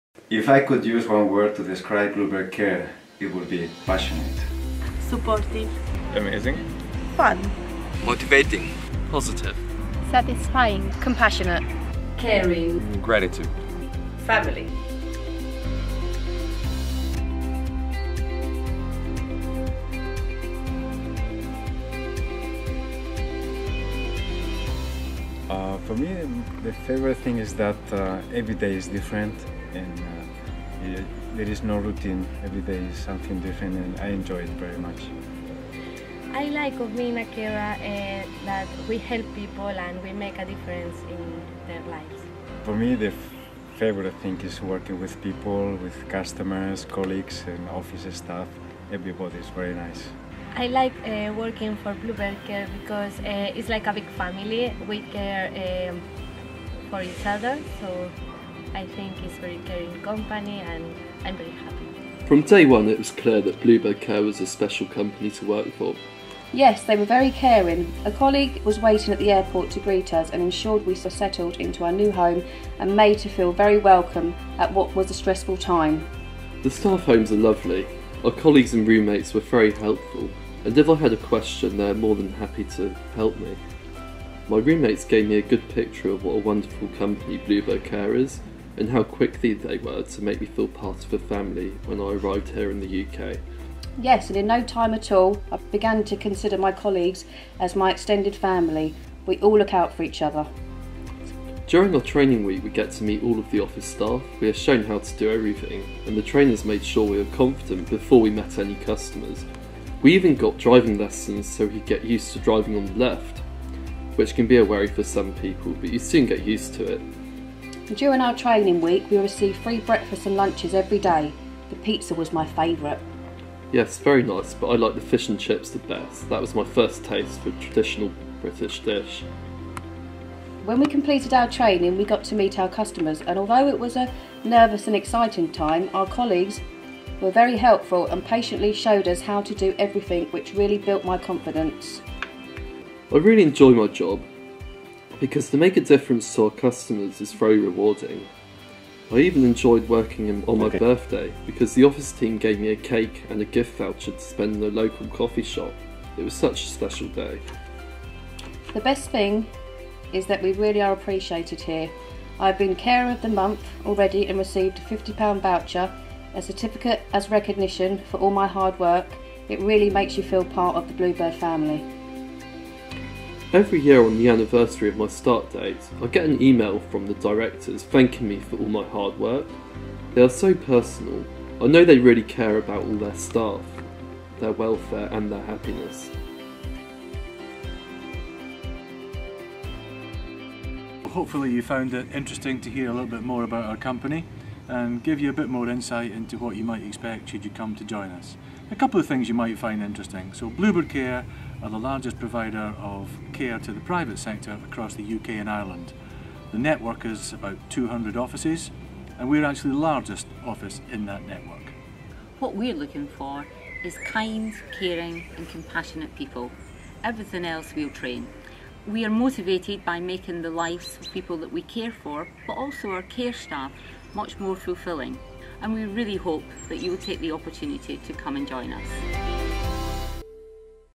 Bluebird Care Recruitment Video
Bluebird Care Slough South Bucks and South Wycombe are recruiting, we are looking for kind, caring and compassionate people and offer excellent training and an excellent package for the right employees. Please get in touch if you are interested in joining our team.